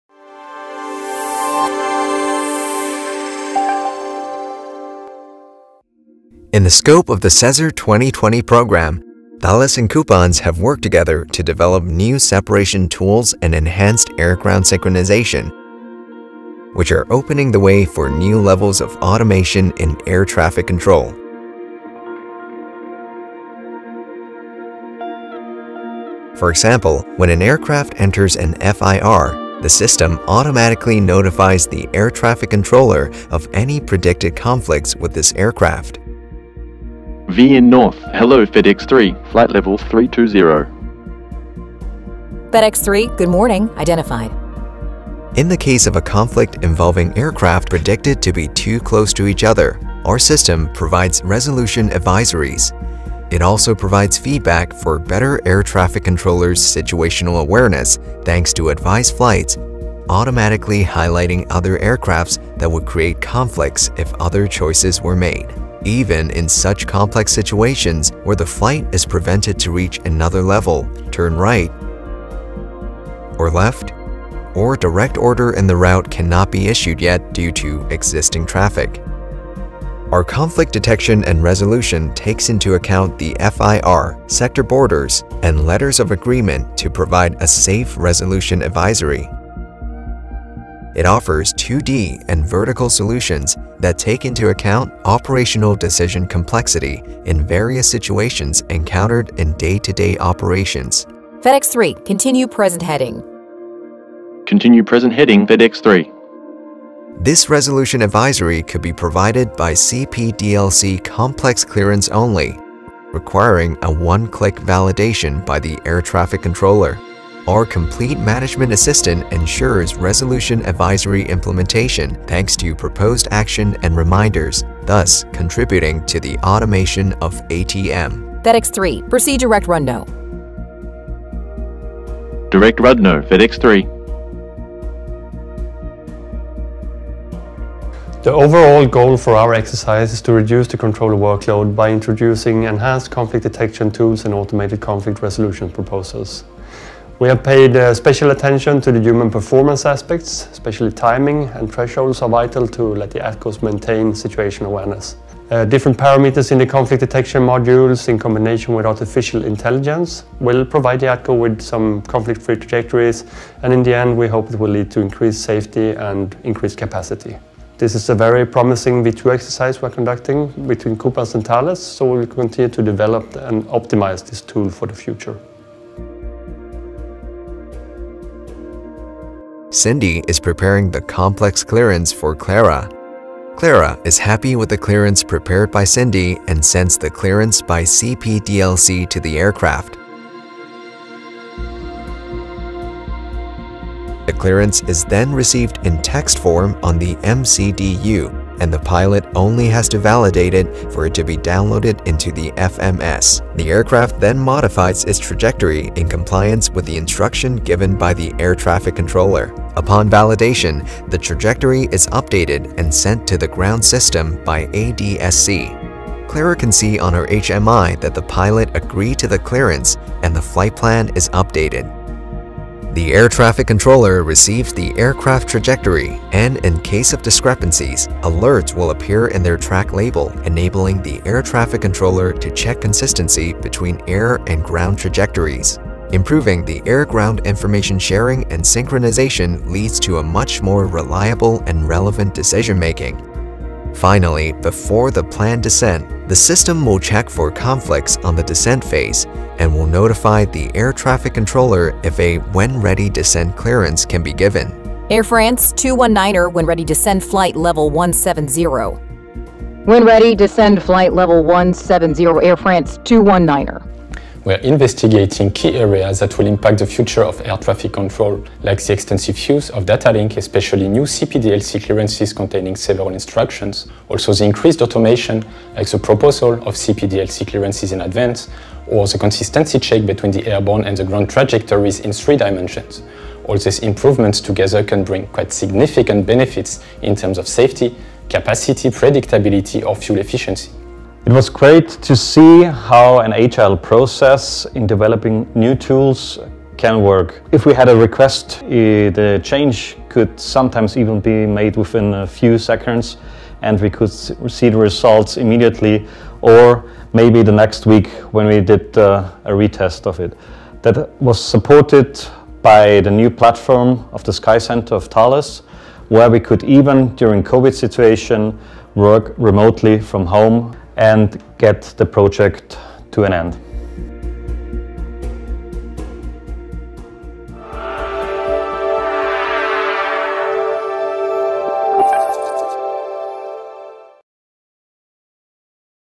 Successful SESAR validations, Trajectory Based Operations
In our role of ATM system supplier for COOPANS Alliance, the leading example in Europe of an industrial partnership with cooperation between multiple air navigation service providers (ANSP), Thales provided the site to perform the SESAR3 Joint Undertaking PJ18 4DSkyways real-time simulations at our SkyLab - France.

Our goal is to reduce the controller workload by introducing enhanced conflict detection and resolution tools, both horizontally and vertically, that will, in combination with artificial intelligence, provide ATCOs with a conflict-free trajectory and thus lead to increased safety and capacity.

Furthermore, we are proud to be addressing - and even anticipating - the upcoming paradigm shift in the involved operational procedures and working methods by investigating the intensive use of CPDLC complex clearances by the Executive Controller or the Planning Controller which will pave the way for further automation, as well as the integration of EPP monitoring in the ATCO radar display.